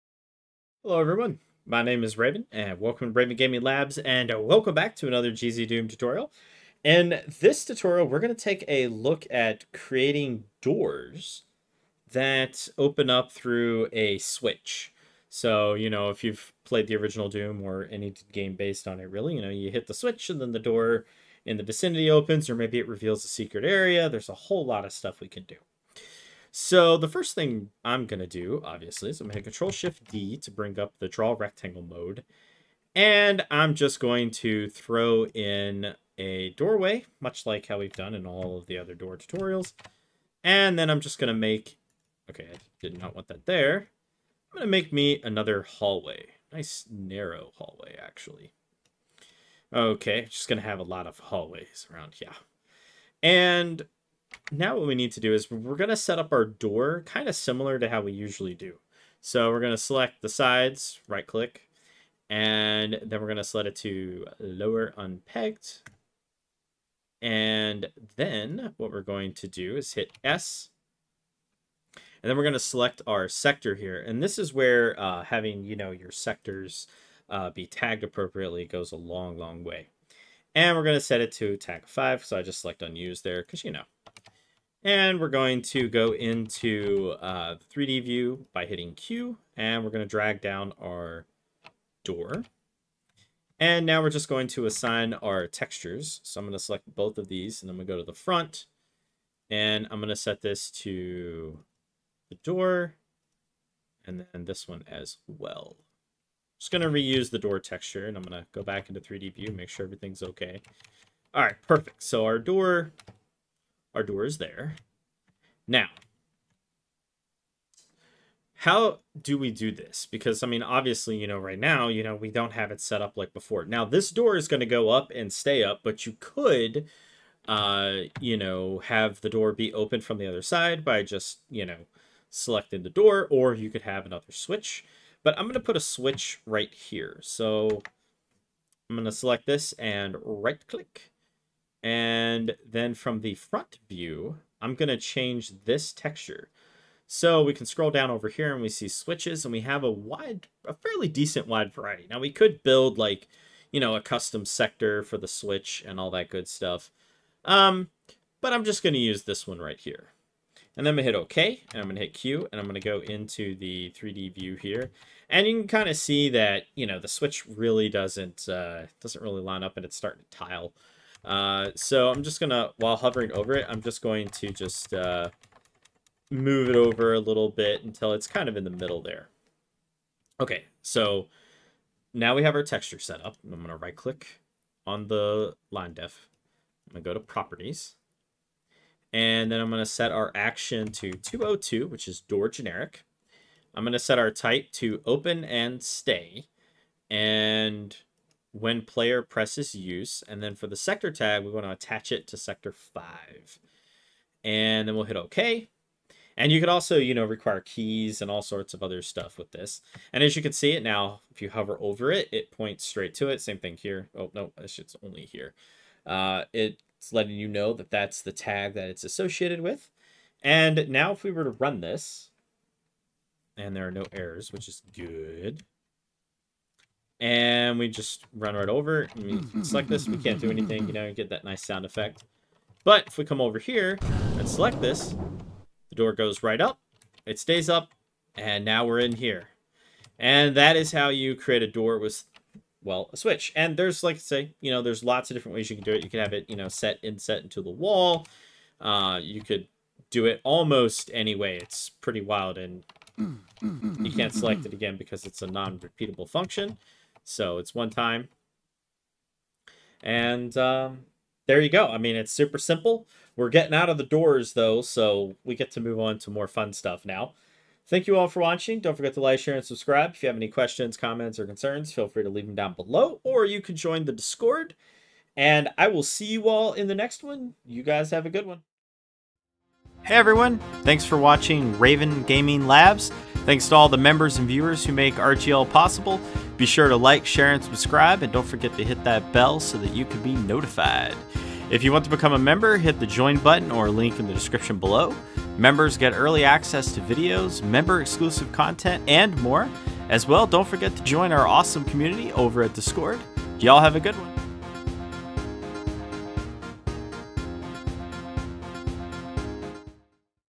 GZDoom Mapping - Doors 4 - Switch Doors - Tutorial 8 | Ultimate Doom Builder(UDB)
In This GZDoom/UltimateDoomBuilder tutorial we will take a look at creating doors that open with a switch.

*** Chapters ***
00:00 - Intro
00:30 - Setting Up The Door
03:40 - Testing The Door
04:20 - Outro